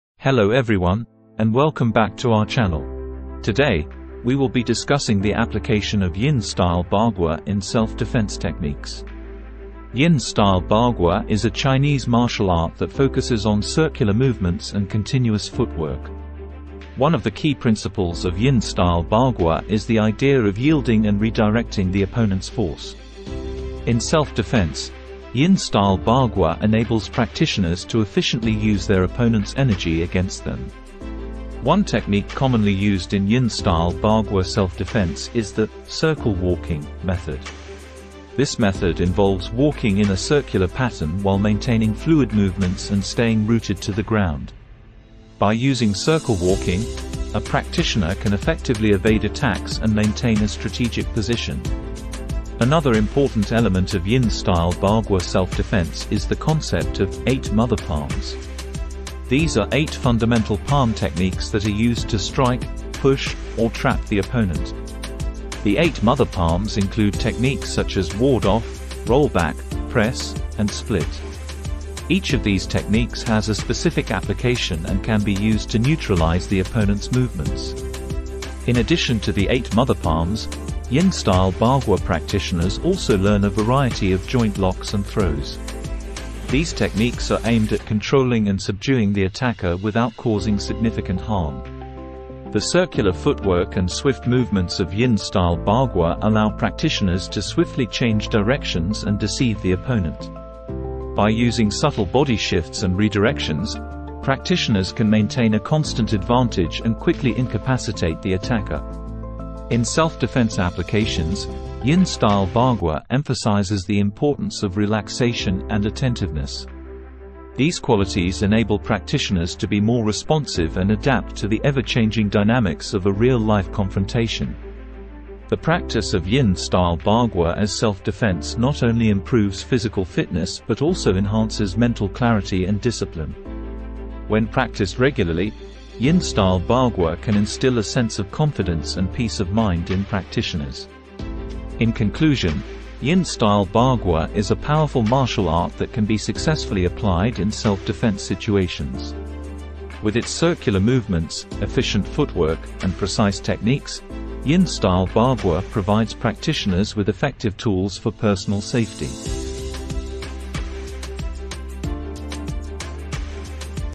Unlocking the Secrets: Yin-Style Bagua for Powerful Self-Defense
Welcome to "Unlocking the Secrets: Yin-Style Bagua for Powerful Self-Defense." In this video, we will explore the art of Yin-style bagua and how it can be applied in self-defense situations.

Yin-style bagua is a traditional Chinese martial art known for its circular footwork and palm techniques. It focuses on using the opponent's energy against them, making it an efficient and effective self-defense system.

Let's start by understanding the principles of Yin-style bagua. It emphasizes the cultivation of internal energy, or Qi, combined with fluid movements and strategic positioning.

One of the key concepts in Yin-style bagua is the Eight Mother Palms. These palm techniques allow practitioners to effectively strike, block, and redirect the opponent's attacks.

The circular footwork of Yin-style bagua gives practitioners the ability to quickly change directions, evade attacks, and maintain balance. It allows for seamless transitions and adaptability in any self-defense situation.

The art of Yin-style bagua is not just about physical techniques but also about mental clarity and awareness. Practitioners learn to observe and analyze their surroundings, making split-second decisions to stay one step ahead of their opponent.

Now, let's see Yin-style bagua in action. Watch as the practitioner effortlessly moves in a circular pattern, deflecting incoming strikes while counter-attacking with precise palm strikes. Each movement is executed with speed, precision, and power.

By practicing Yin-style bagua, you can develop increased strength, flexibility, and coordination. It is a martial art that not only empowers you physically but also instills confidence and discipline.

So, if you are looking for a comprehensive self-defense system that combines fluid movements, strategic footwork, and internal energy cultivation, Yin-style bagua is the answer. Discover the secrets of this ancient martial art and unlock your true potential.

In this captivating video, we delve into the intriguing world of Yin-style Bagua as utilized in self-defense scenarios. Prepare to unlock the secrets of this renowned martial art that has fascinated practitioners for centuries.

Discover the artistry, grace, and power intertwined within Yin-style Bagua. Our expert instructors will guide you through the fundamental techniques, emphasizing the practical applications and self-defense aspects of this ancient martial art form.

Learn how to harness the circular footwork, intricate palm strikes, and swift evasive maneuvers that make Yin-style Bagua such a formidable self-defense system. Gain insights into the philosophy behind this art, as we explain how focus, intuition, and adaptability contribute to its effectiveness in real-world confrontations.

Whether you are a martial arts enthusiast seeking to expand your knowledge or an aspiring practitioner looking to enhance your self-defense skills, this video is a must-watch. Join our passionate community as we explore the depths of Yin-style Bagua and its practical applications for personal protection.

Unlock the secrets of Yin-style Bagua and embark on a transformative journey towards mastery and self-discovery. Watch now and find true inspiration for your martial arts practice.